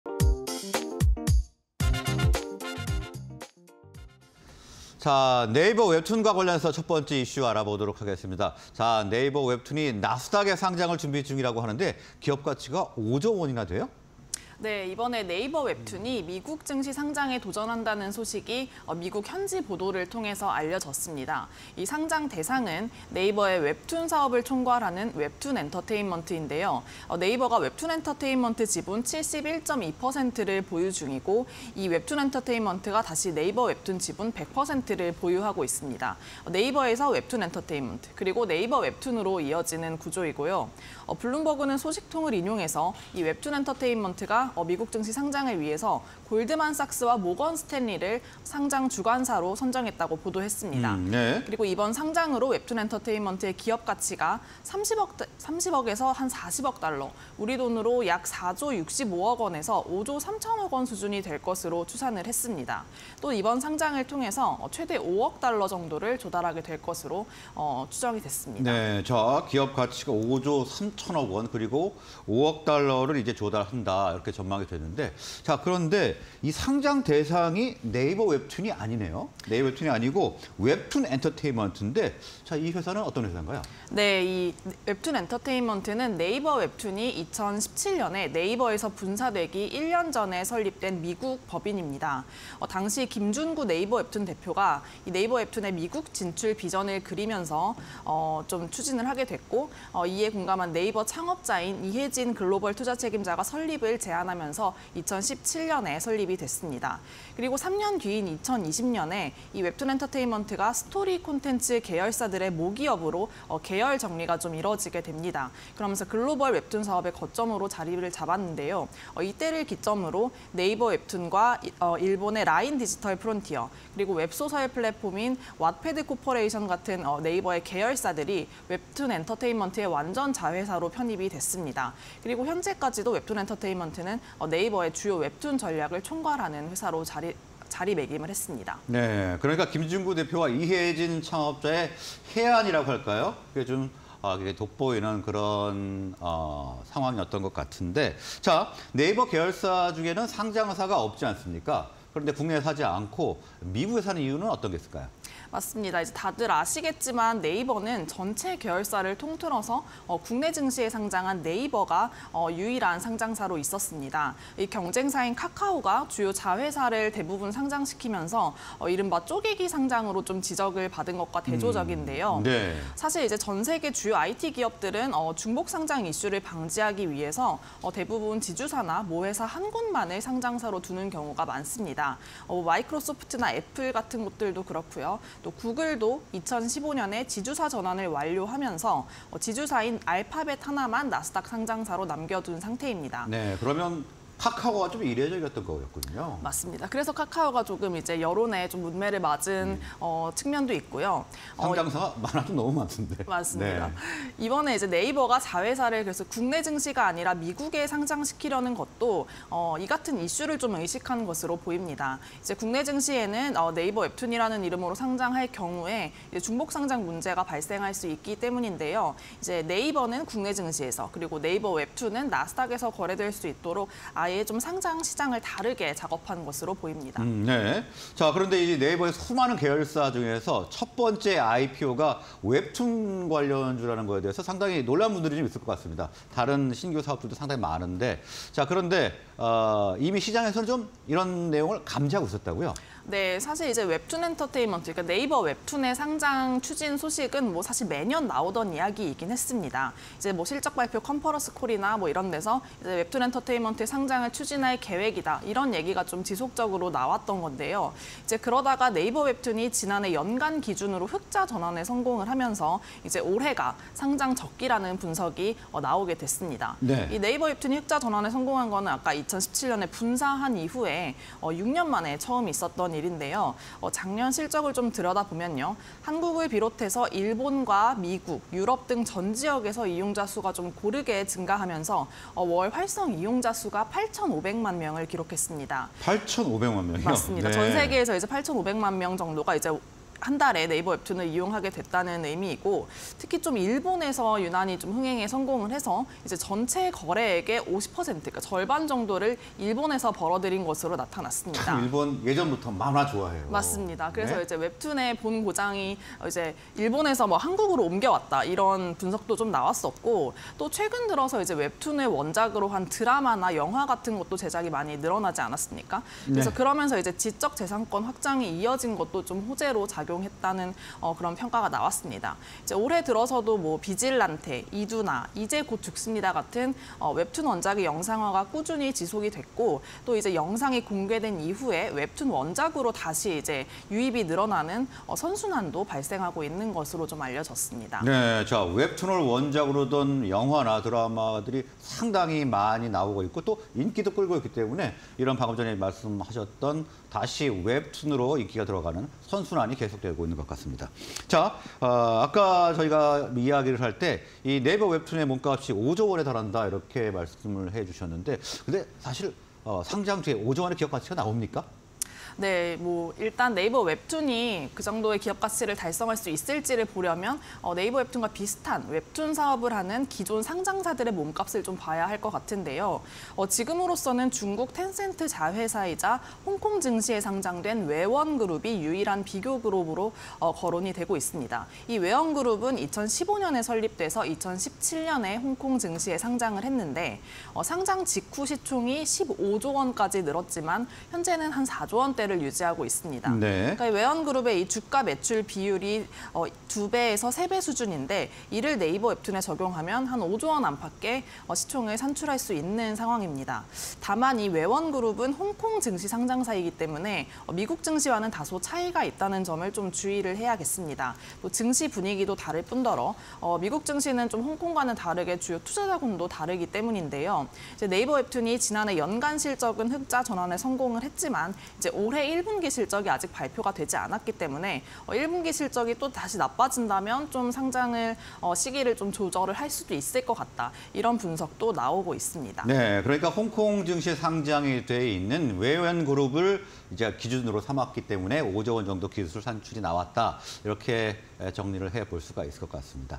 나스닥 향하는 '네이버웹툰'... 기업가치 최대 5조원 가능할까?_기자들의 백브리핑 (20240222)
Q. "나스닥 갑니다" 네이버웹툰
Q. 웹툰엔터테인먼트의 주요 사업은?
Q. 네이버웹툰, 왜 미국서 상장할까?
Q. 네이버웹툰, 美 상장 '성큼'... 시장 반응은?
Q. '美 상장 시동' 네이버웹툰 기업가치는?